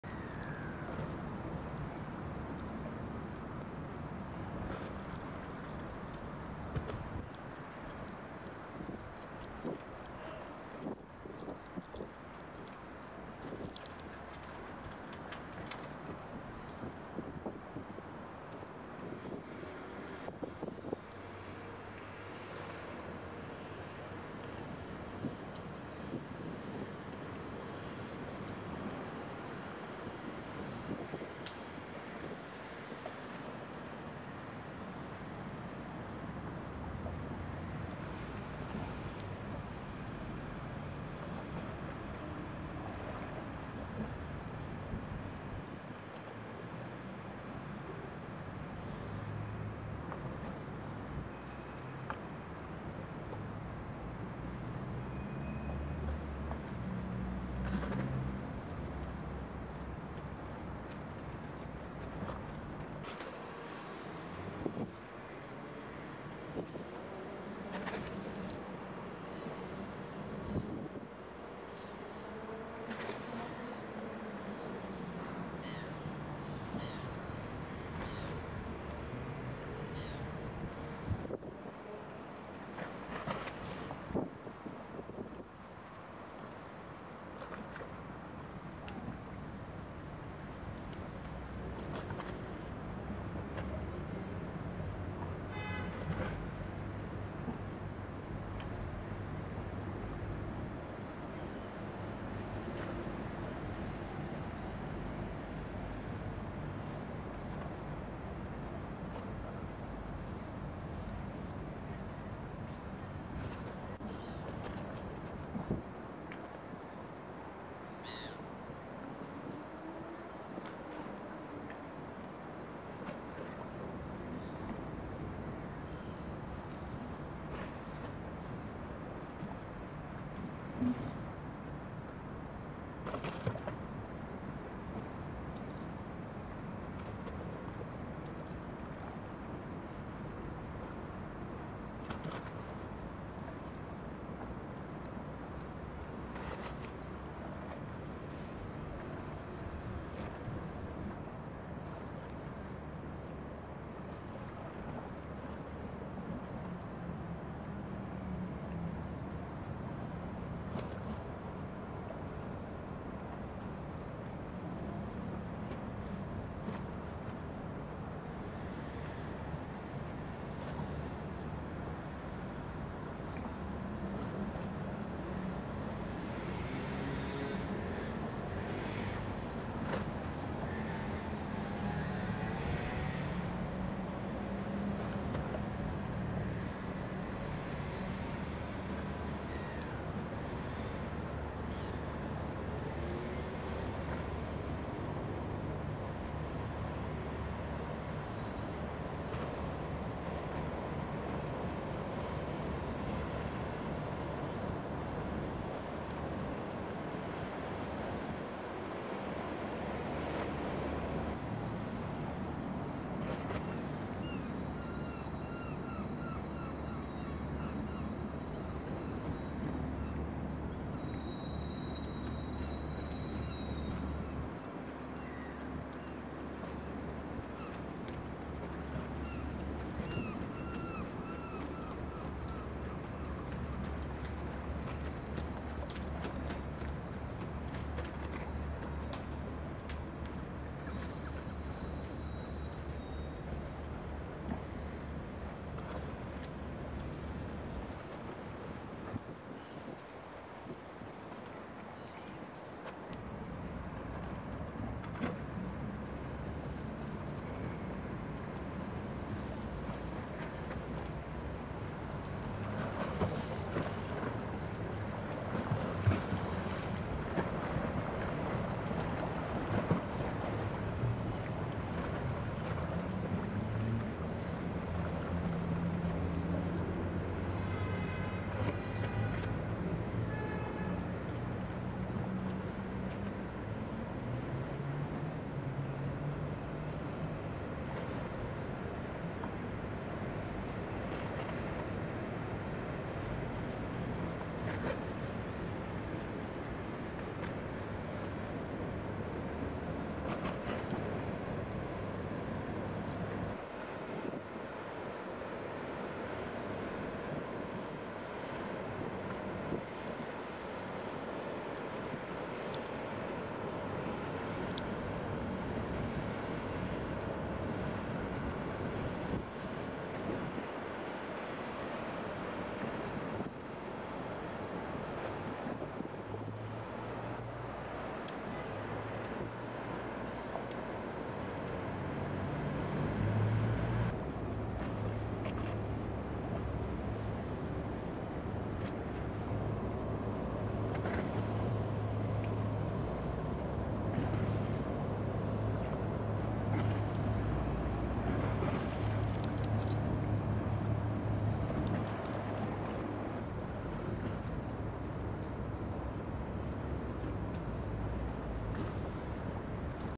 VIKING VENUS CRUISE SHIP PASSING THAMES BARRIER
VIKING VENUS cruise ship passing the Thames Barrier near Woolwich on 13th August 2022.

Viking Venus was on a 14 night one way cruise from Greenwich to Stockholm, Sweden and her next port of call was Dover.